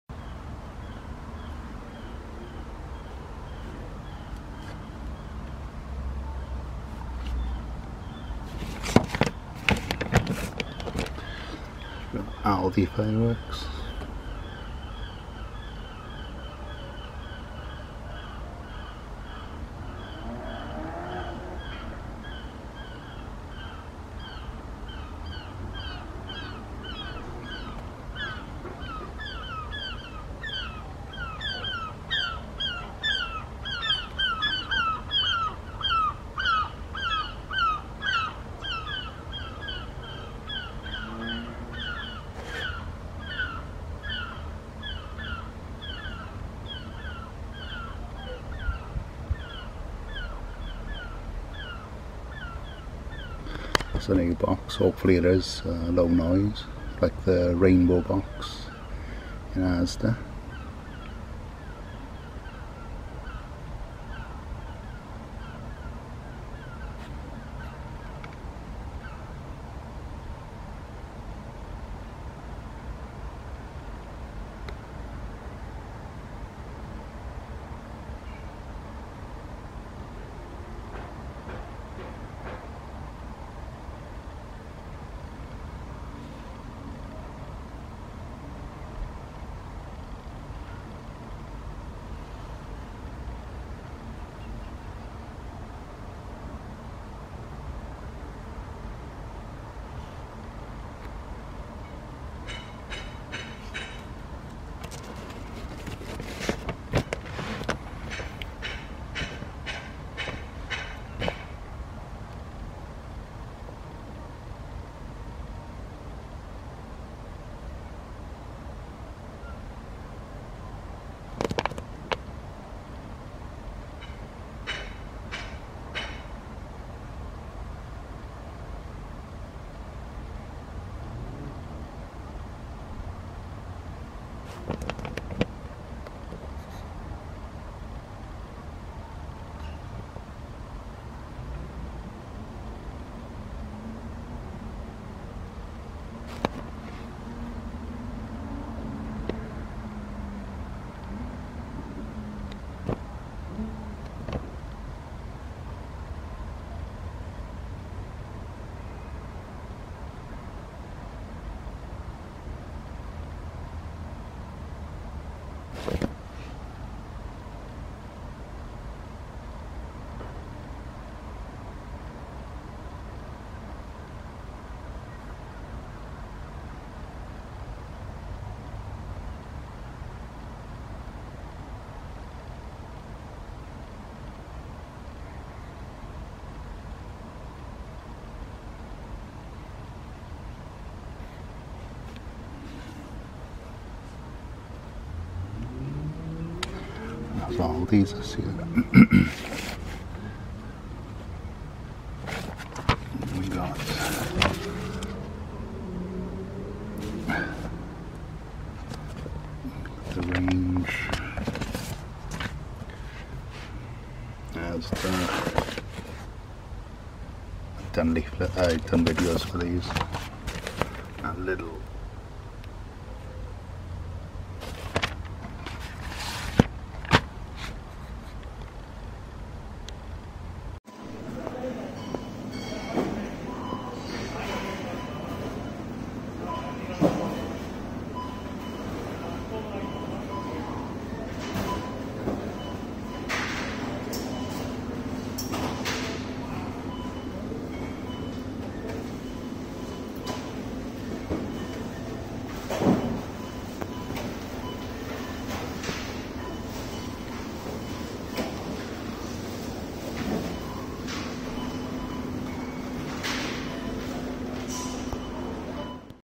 Aldi fireworks 2021
A  look at their fireworks for this year , and a quick look at the lidl firework display unit instore.